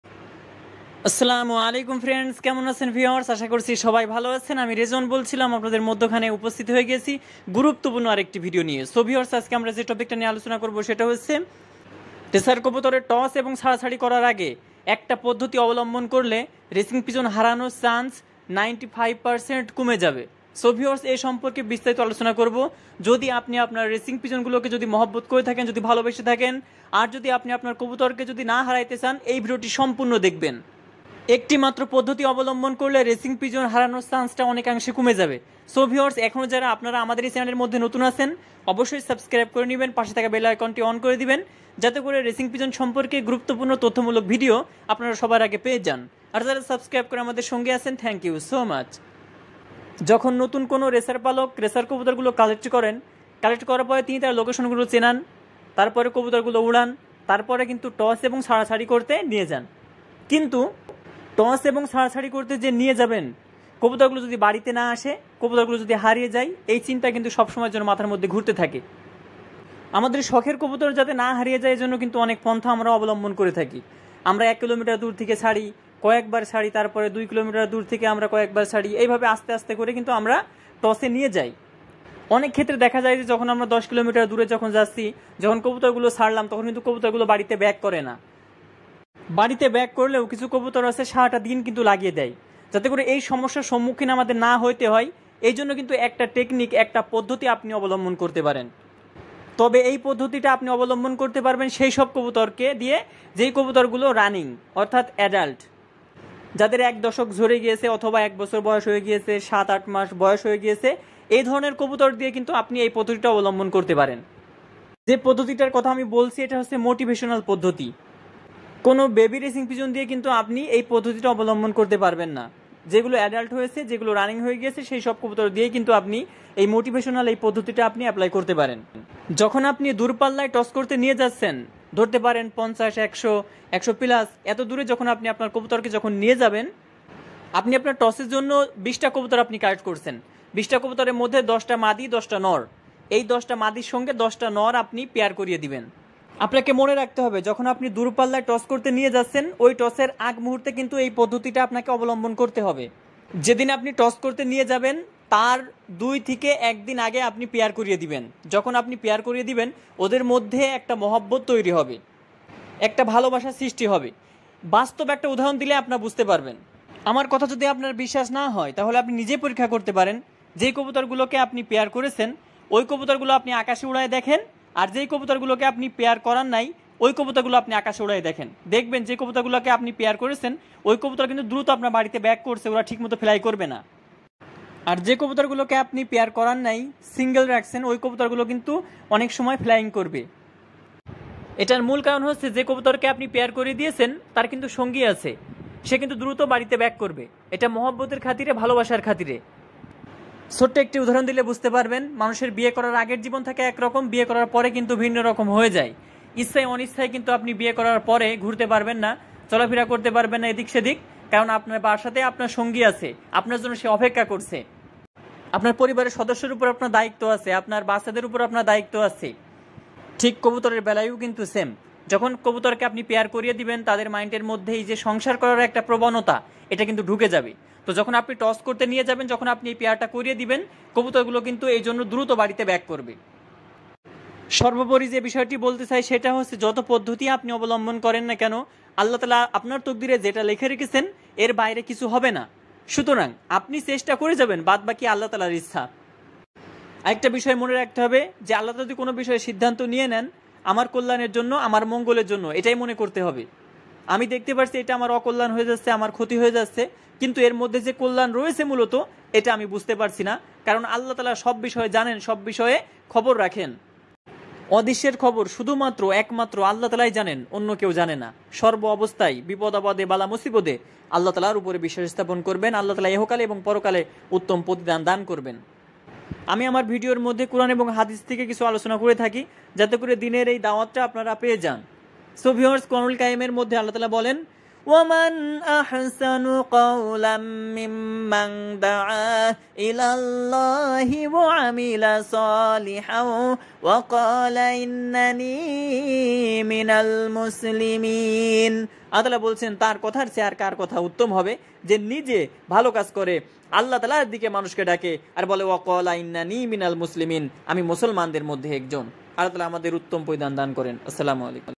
রেসার কবুতর হারানোর ভয় আর না। রেসার কবুতরের মোটিভেশনাল পদ্ধতি।
আসসালামু আলাইকুম